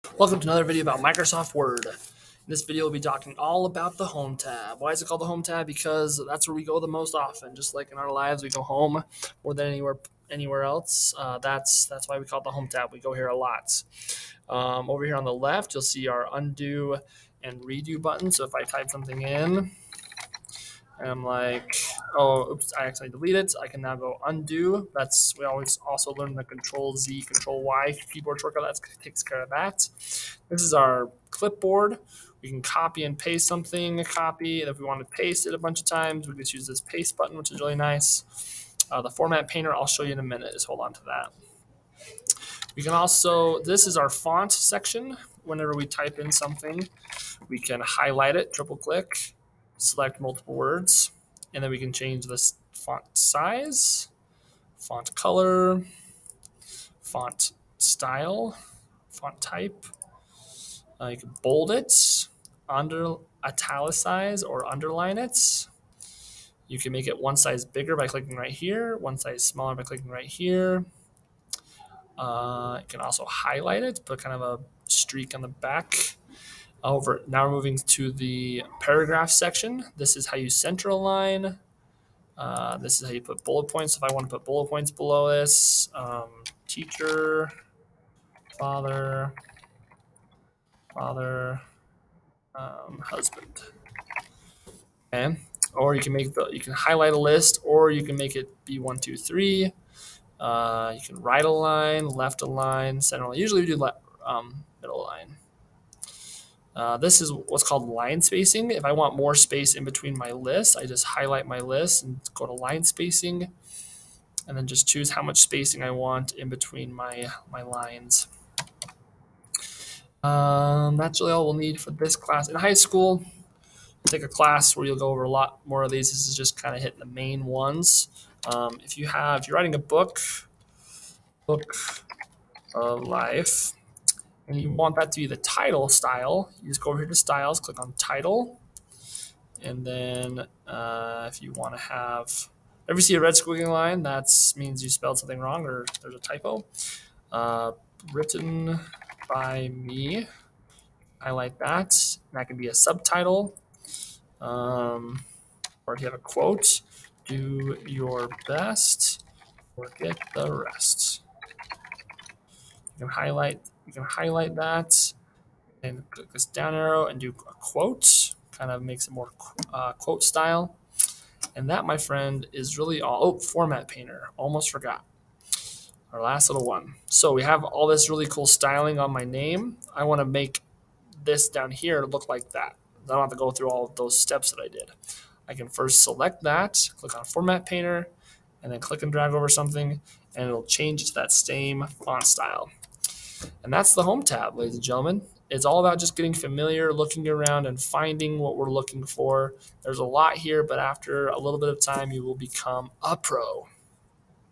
Microsoft Word -- Home Tab 👍
Come along and join your tour guide, Mr. Swallow, as he explores the Home tab. A trilling adventure you won't want to miss. :)